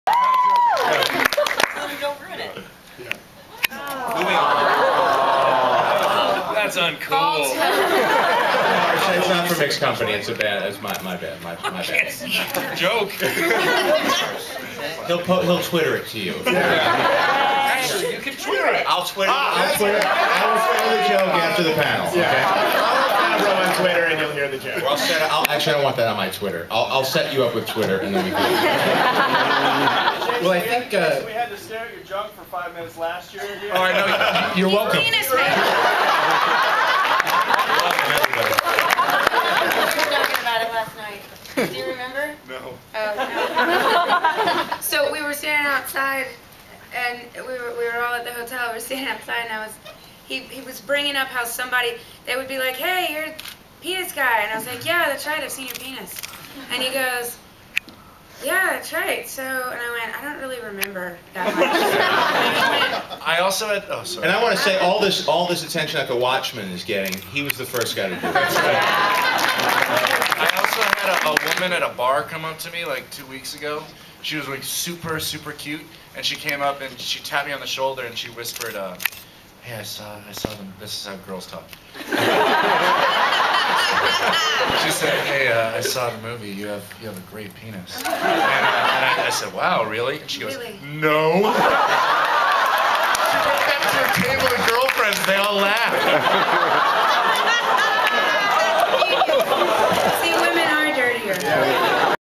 I Love You Man Panel @ SXSW
Great comedy from Paul Rudd, Jason Segel, Jon Favereau, Rashida Jones, and Jaime Pressley, as they talk about filming the movie "I Love You Man".